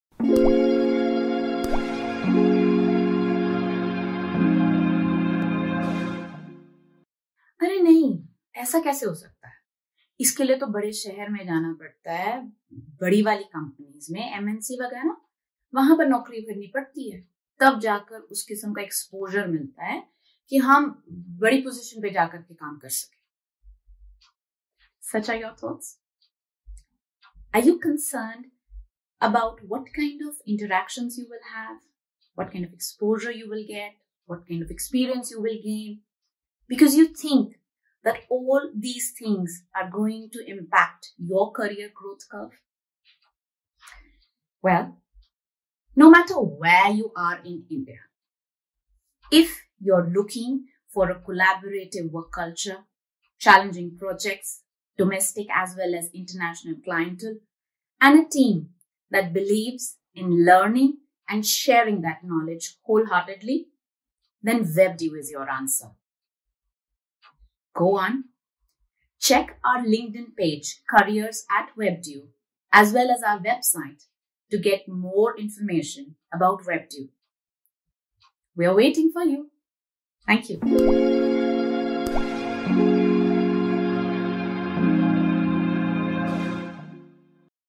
Webdew Myth Busters: Exposure
It a common believe in the industry that only MNCs and big Companies can provide you the exposure you need to grow in your career.

We invite you to visit webdew and change your mind forever!
Our growth oriented mindset is all about helping our team members scale upwards and achieve new milestones.